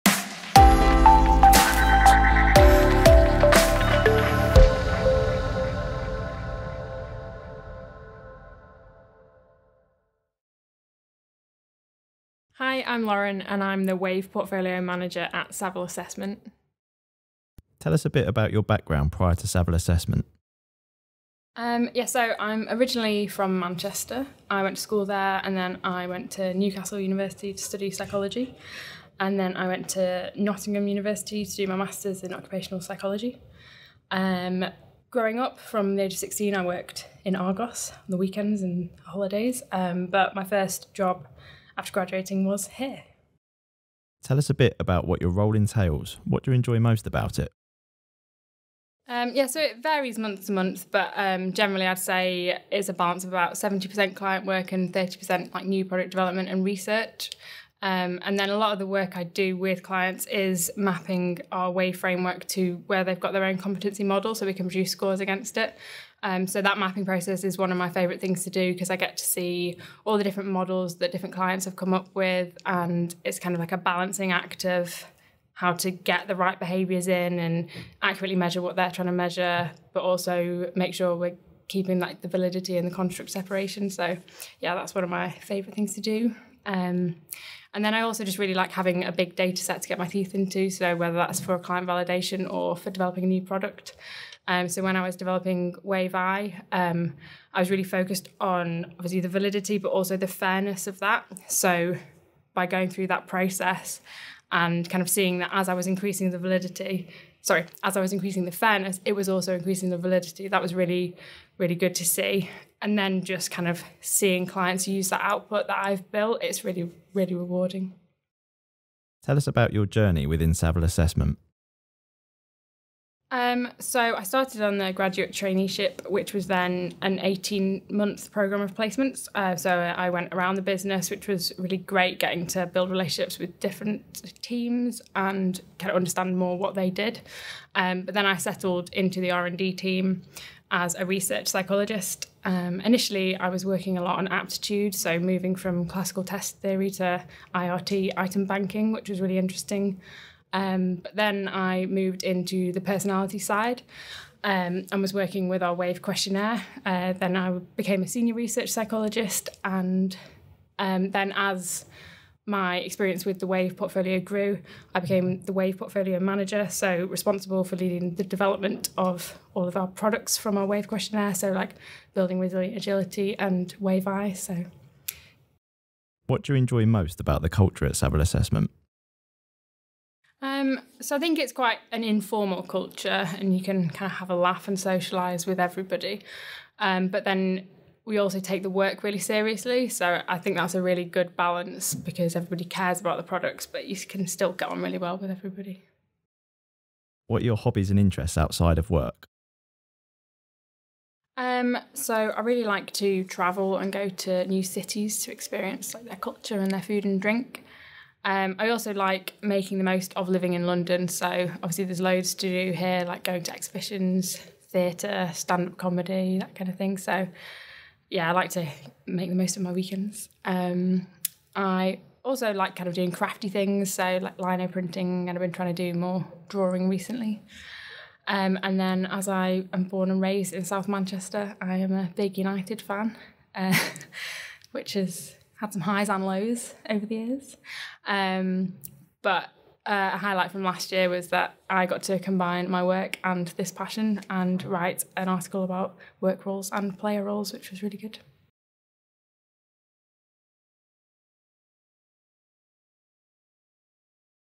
Staff Spotlight - Lauren
What's it like to work at Saville Assessment?

0:00 Intro
0:18 Background Prior To Saville Assessment
0:44 Tell Us About Your Role?
1:59 Your Journey Within Saville
2:56 The Culture At Saville
3:17 Hobbies/Interests
4:09 Connect With Lauren